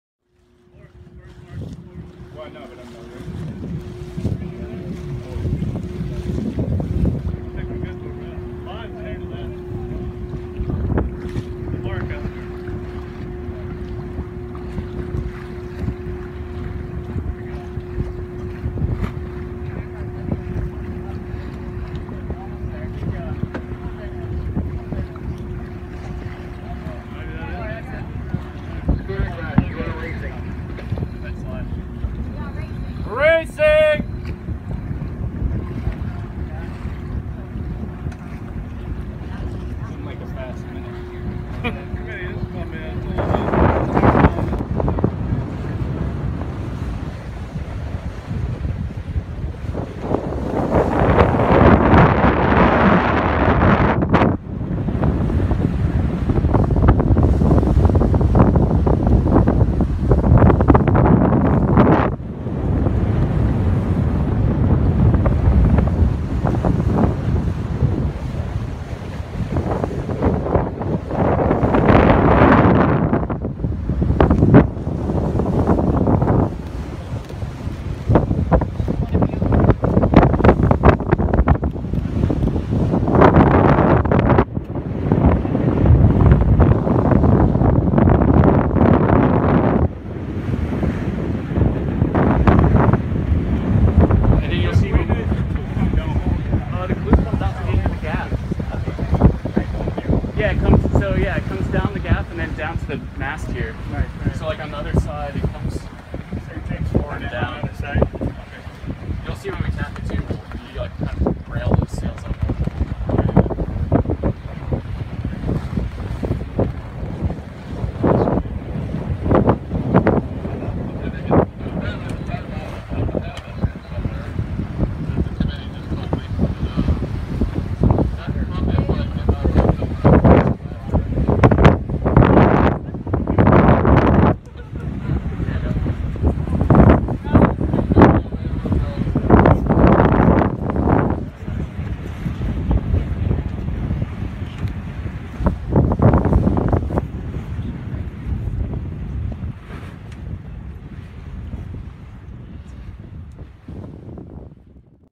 Schooner Columbia - 2017 Morgan Cup Winner
Schooner Columbia racing in the 2017 Morgan Cup
CT Maritime Heritage Festival
September 10, 2017
Long Island Sound, New London Connecticut